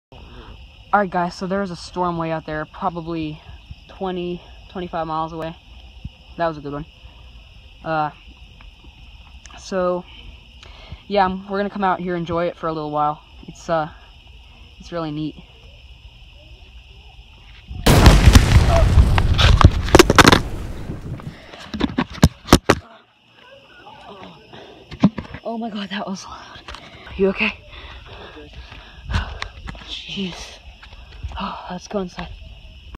INSANELY CLOSE LIGHTNING STRIKE!! CRAZY SOUND!!
The only injury I got from this was scraped arm from falling on the pavement LOL